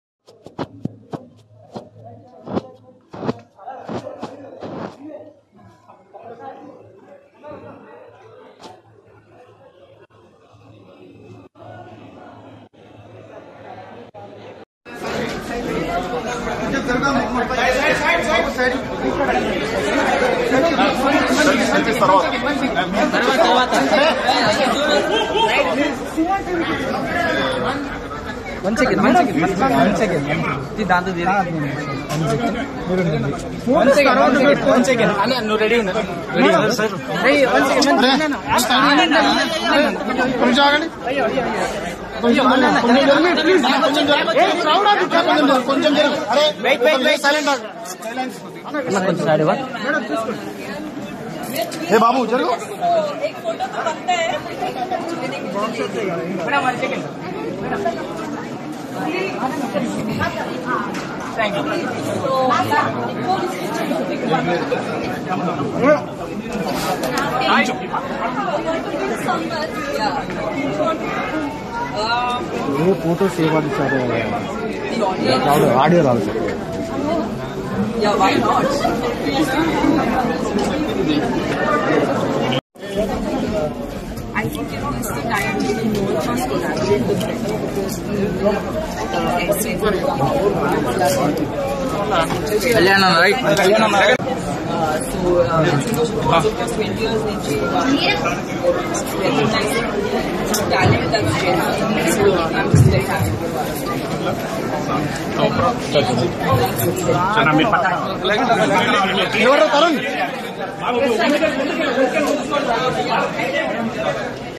Best Actress Tamanna Bhatia | Santhosham SumanTV Awards 2021 | Tamanna Received Award
Best Actress Tamanna Bhatia | Santhosham SumanTV Awards 2021 | SumanTV | Tamanna Received Award